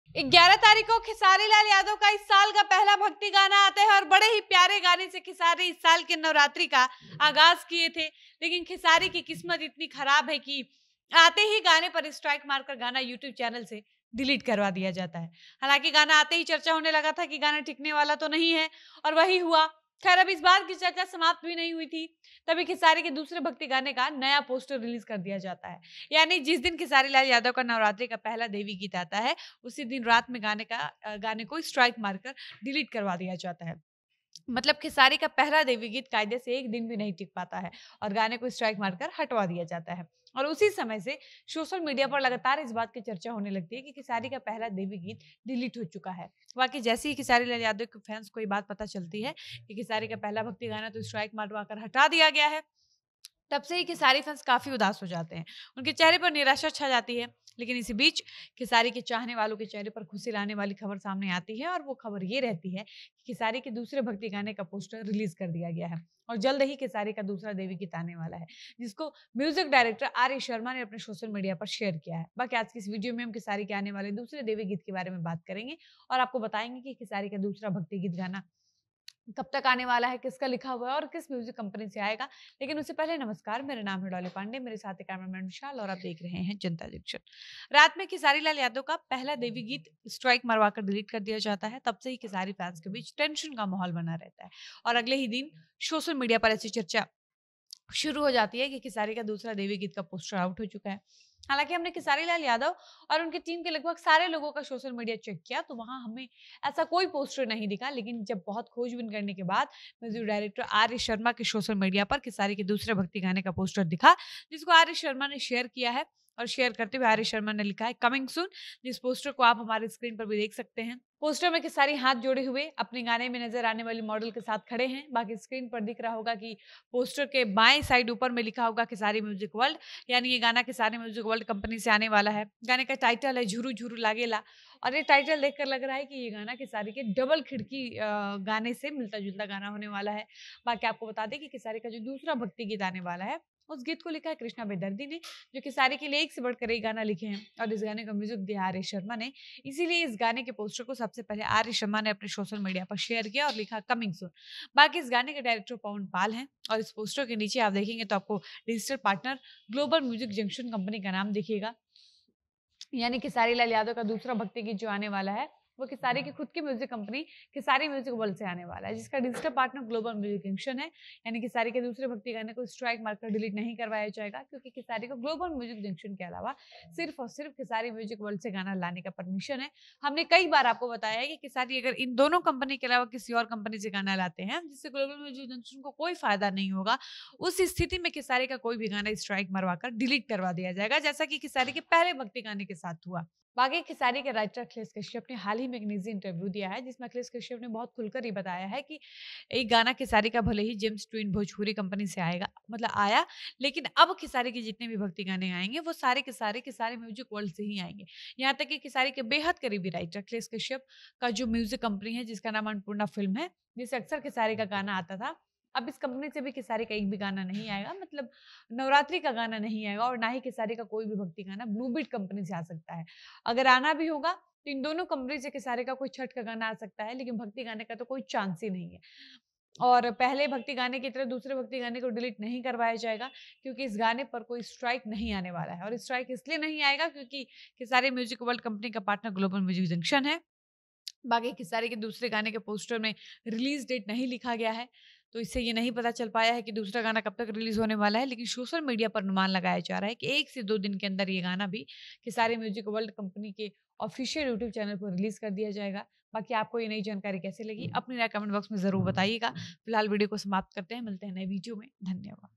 विवाद के बीच Khesari के दूसरे देवी गीत का भी पोस्टर आ गया है। Devi Geet Bhojpuri
विवाद के बावजूद पहला भक्ति गीत डिलीट होने के बाद खेसारी के दूसरे भक्ति गाने का पोस्टर रिलीज हो चुका है, इस बार खेसारी का गाना कोई चाह कर भी डिलीट नहीं करवा सकता है।